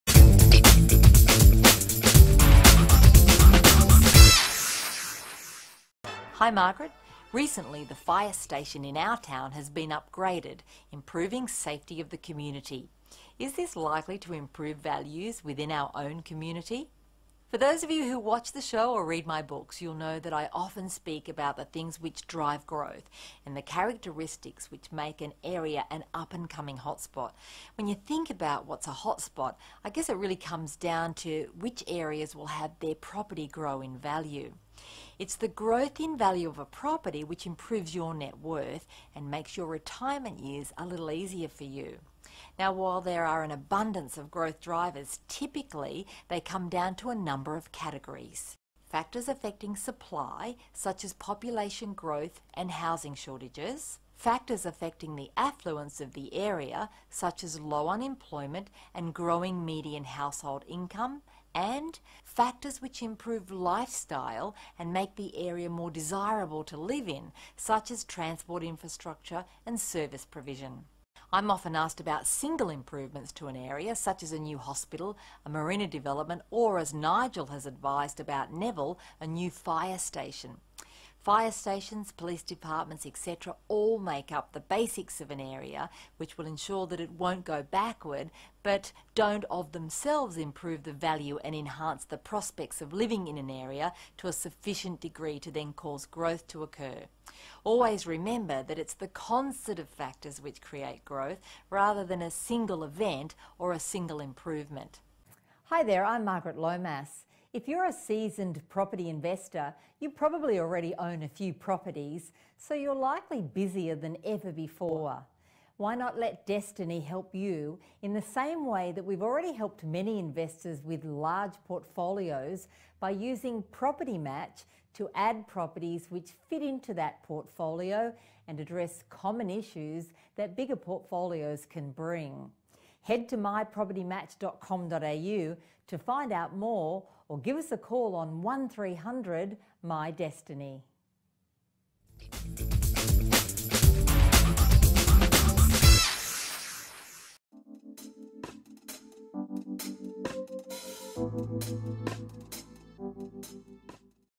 What infrastructure drives growth?
Not all infrastructure drives growth in an area.  Watch this video to see what does!